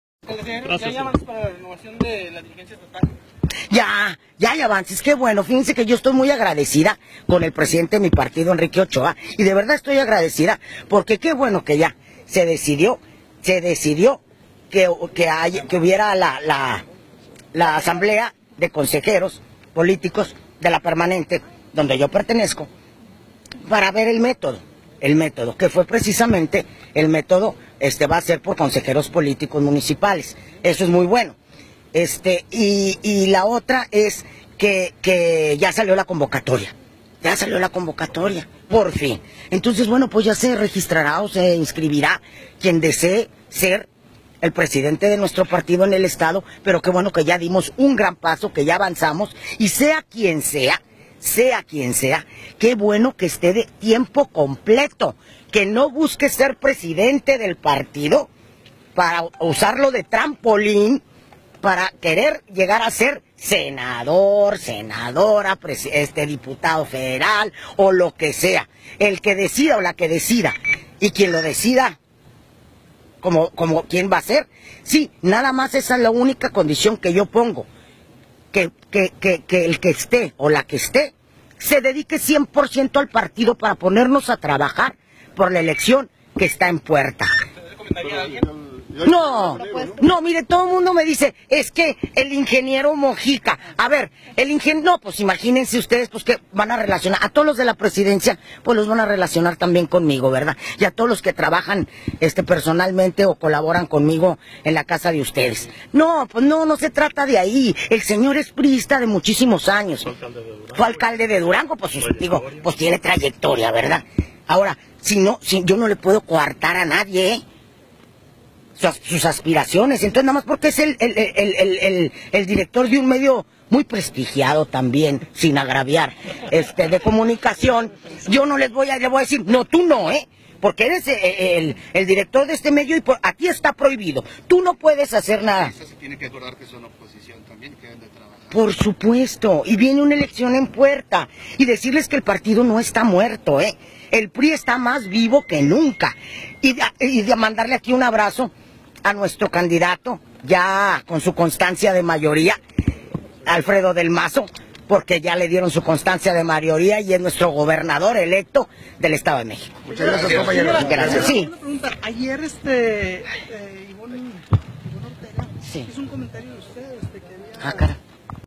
Felicita Leticia Herrera a Enrique Ochoa
Felicita Lety Herrera que por fin hay avances para elegir a la nueva dirigencia estatal del PRI en Durango y exige que quien llegue se comprometa a no buscar una candidatura y usar para ello al partido.